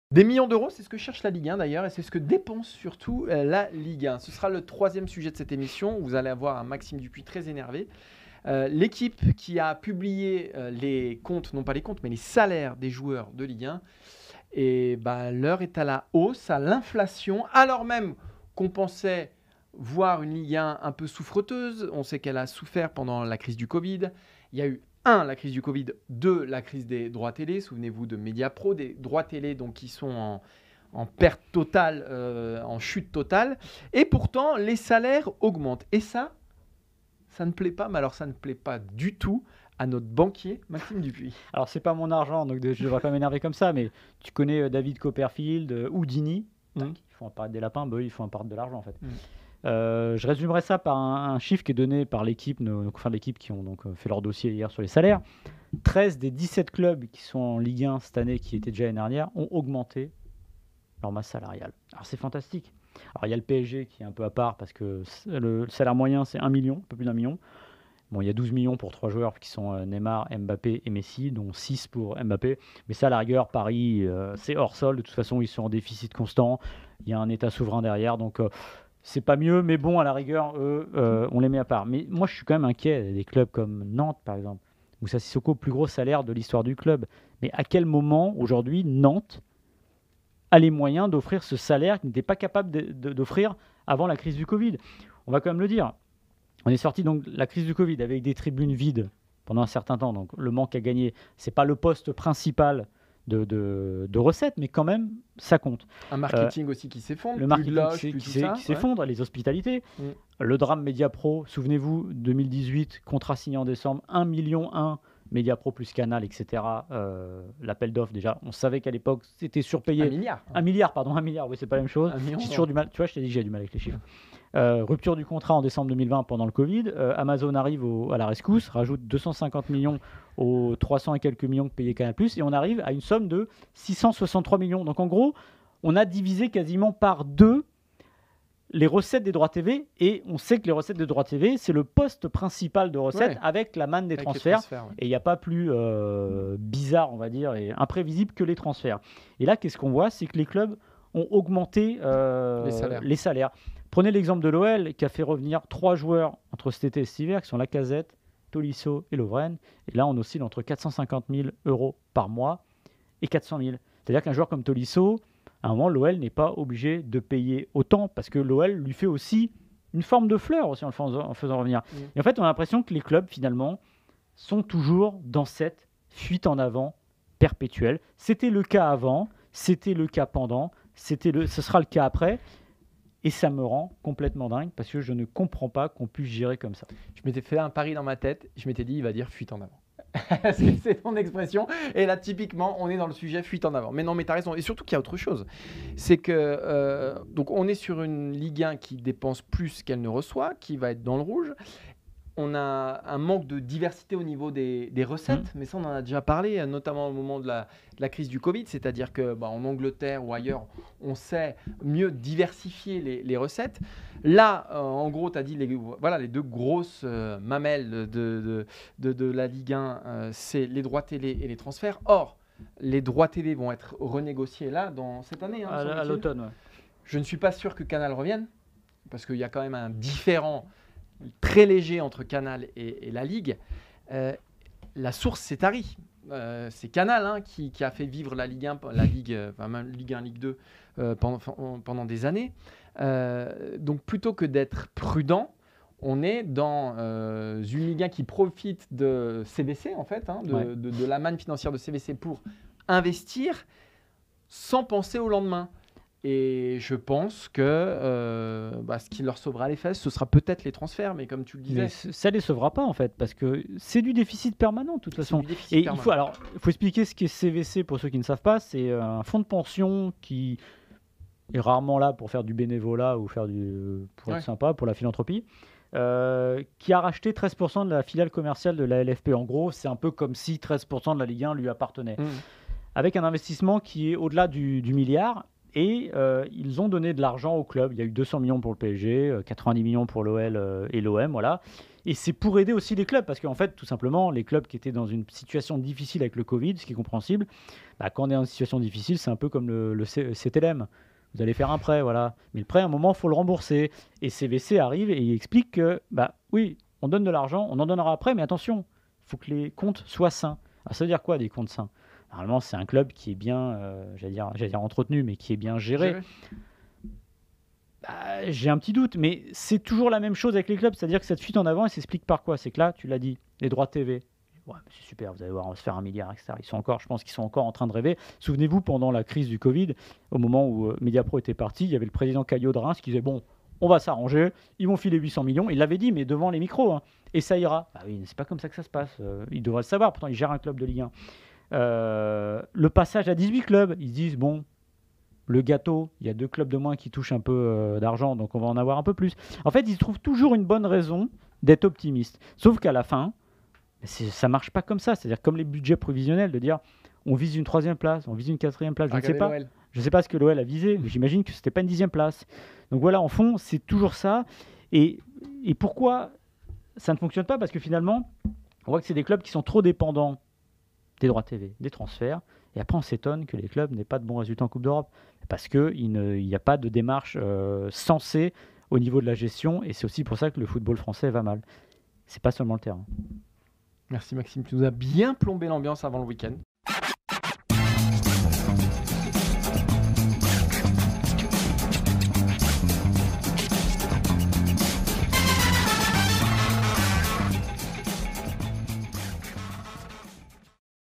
LIGUE 1 - Les salaires de la démesure : Pourquoi le championnat se dirige droit dans le mur
LIGUE 1 - Les salaires des joueurs de L1, révélés cette semaine par L'Equipe, sont globalement à la hausse. Pourtant, la santé financière des clubs de l'élite ne dessine pas la même courbe. Martin Mosnier et Maxime Dupuis relèvent cette incohérence pécuniaire dans cet extrait du FC Stream Team, un podcast à retrouver chaque vendredi. [Réalisation et graphisme : Simon Farvacque et Quentin Guichard]